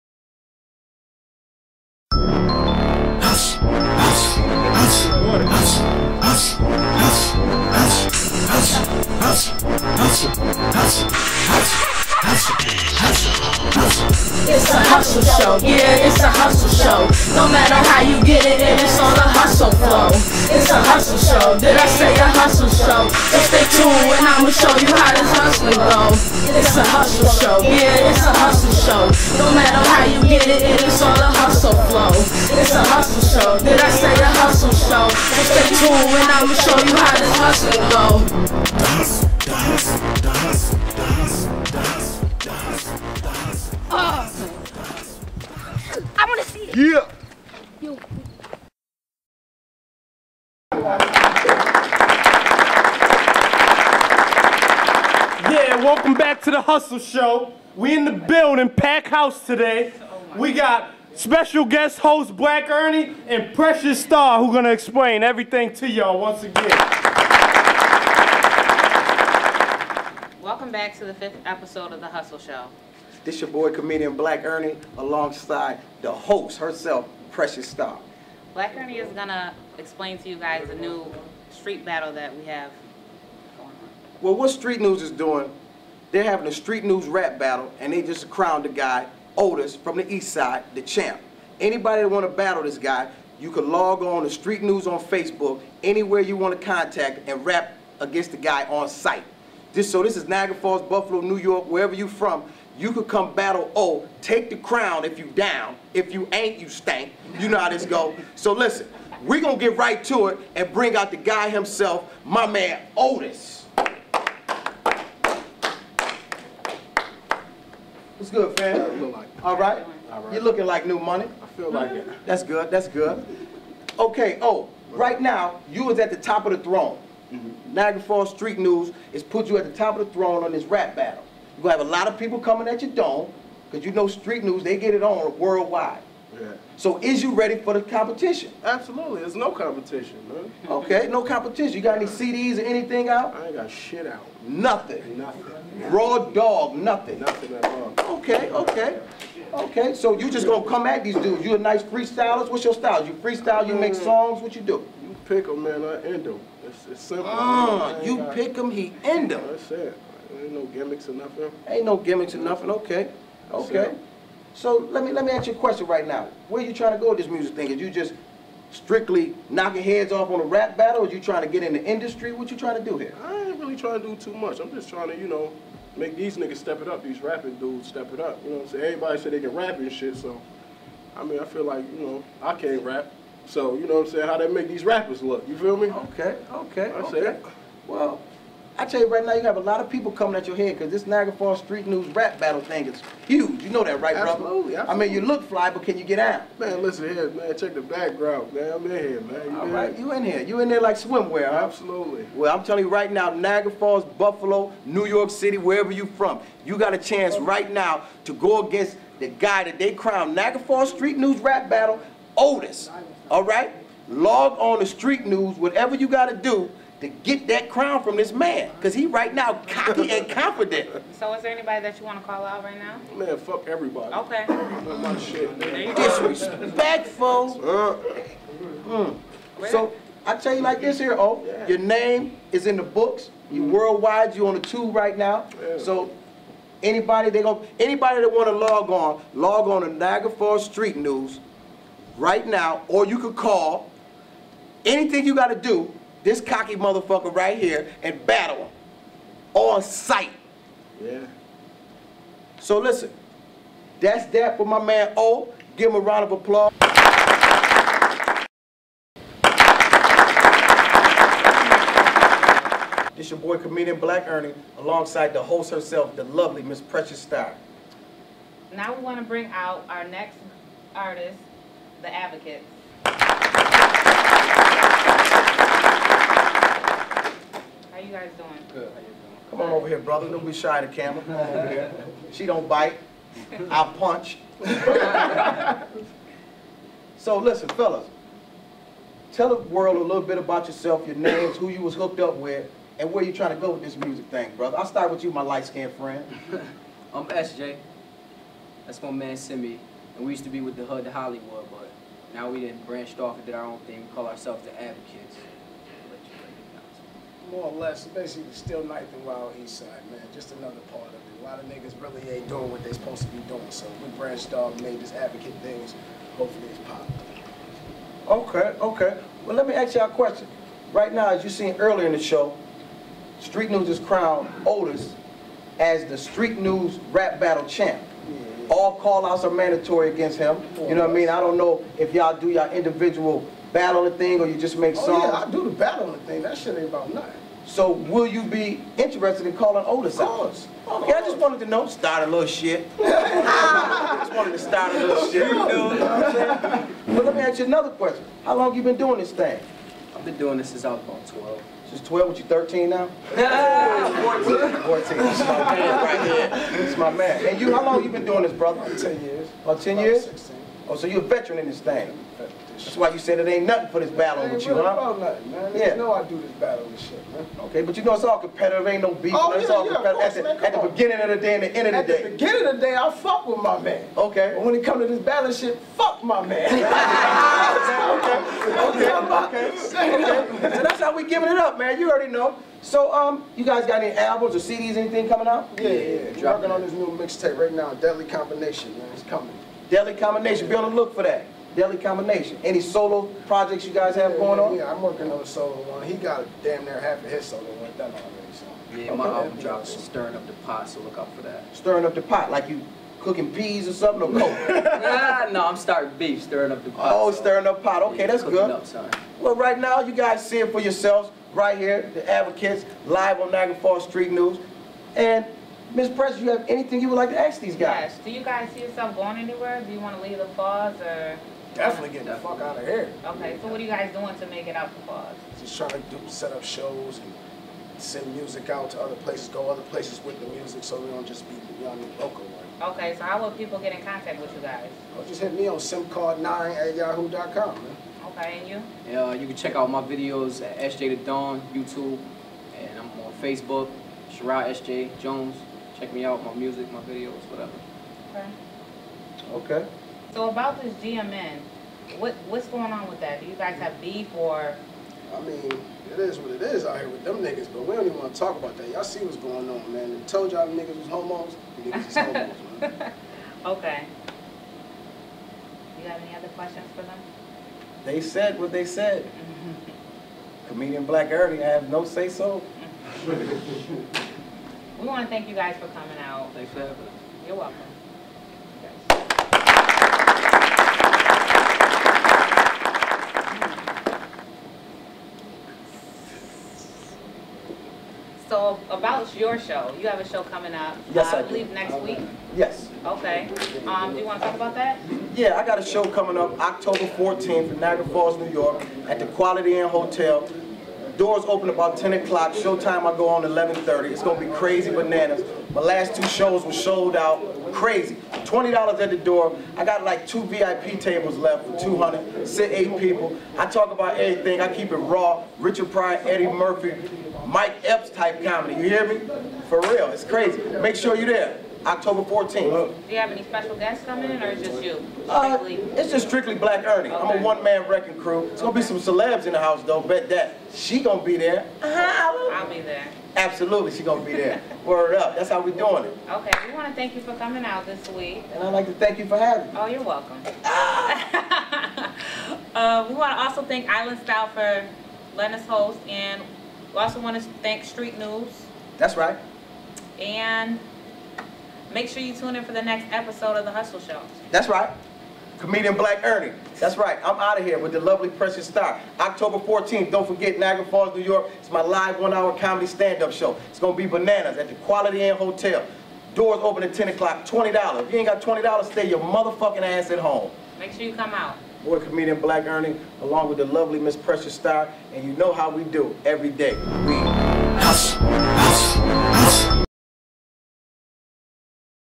The Hustle Show S1:05 Otis, The Advocates and guest host Black Ernie
Season 1, episode 5 of the Hustle Show. Comedic guest host Black Ernie interviews the first street battle crowned king Otis, and two young rappers from the group "The Advocates". The Street battle king Otis challenges the competition by saying there's absolutely NO competition in the Falls, meanwhile the Advocates stir up the pot by speaking on their former group Hood 2 Hollywood and about the recent GMN homosexuality controversy. The show is concluded with Black Ernie speaking to the Hustle Show about his upcoming comedy show at the Quality Inn, watch the entire show to find out more!!!